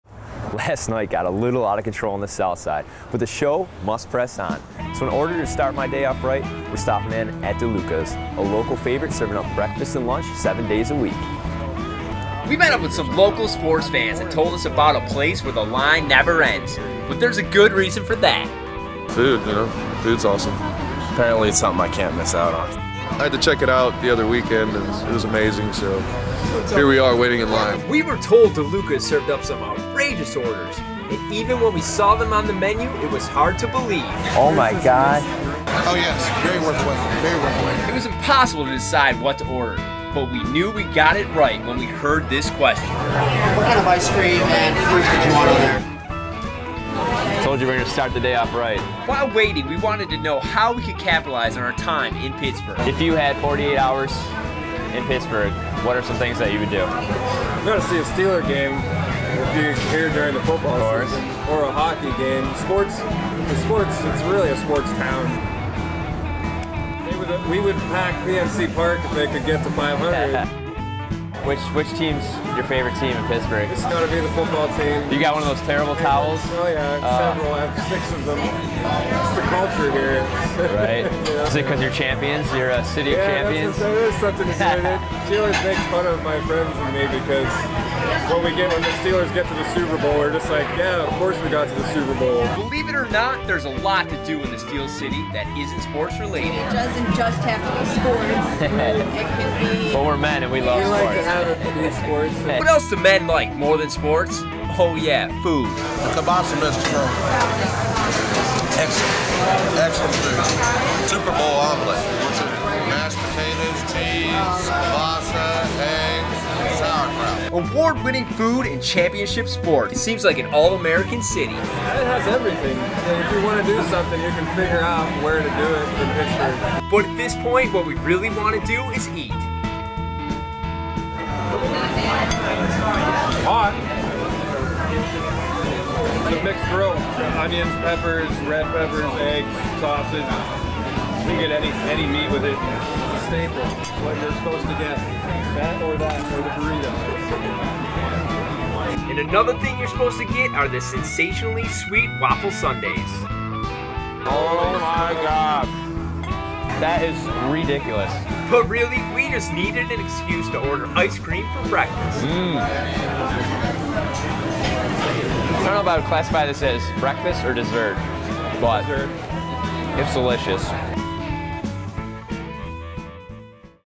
Deluca's Restaurant
Deluca's is arguably the best breakfast joint in Pittsburgh. Not only does it serve up some mouth-water staples, but they take dessert breakfasts to the extreme.